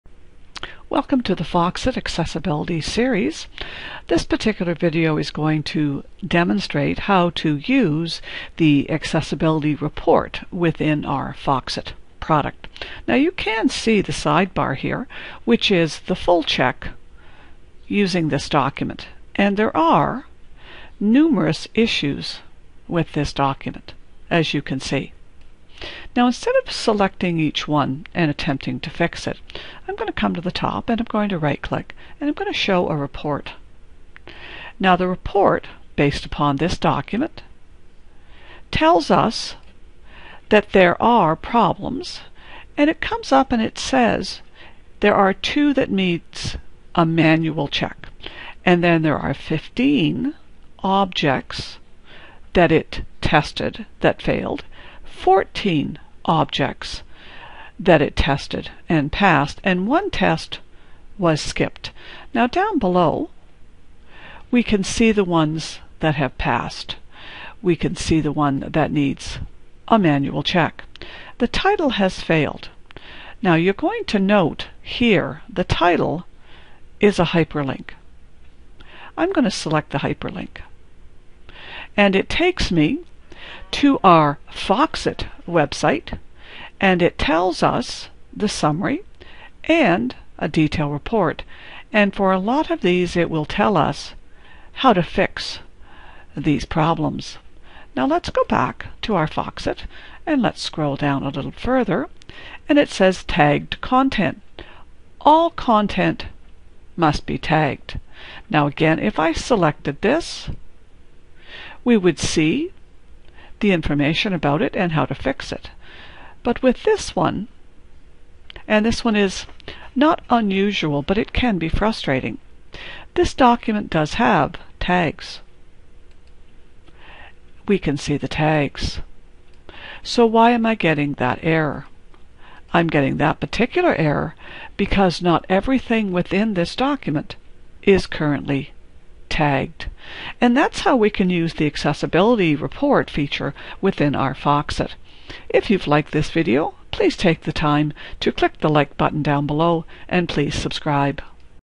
Foxit Accessibility Using the Foxit Accessibility Report
Creating Accessible PDF files using Foxit.   Using the FOXIT Accessibility Report.